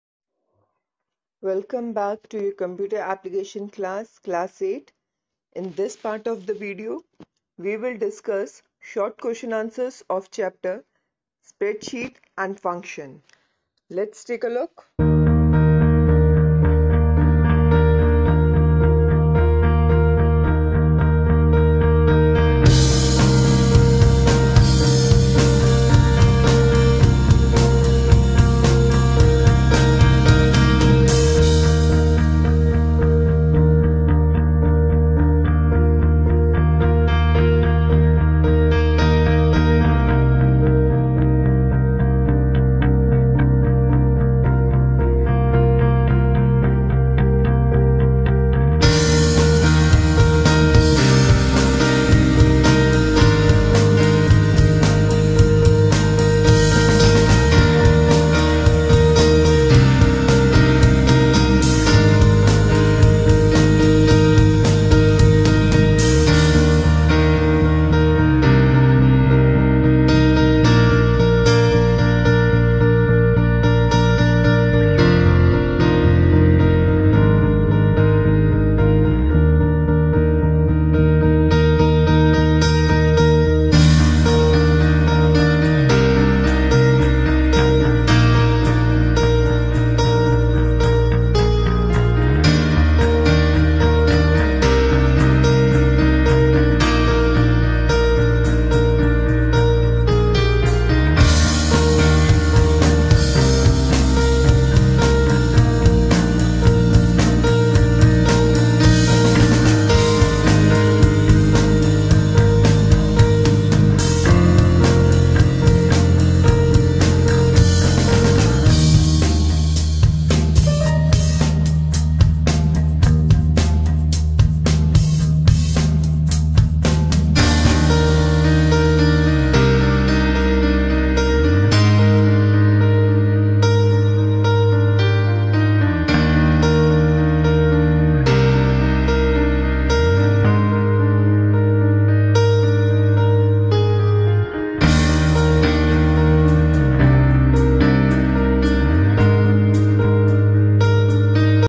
Class-8, Subject-Computer, Video-17, Chapter-Spreadsheet-Functions, Part-4 by Miss Rancy
Class -8, ch- Spreadsheet - Functions, part - 4, video - 17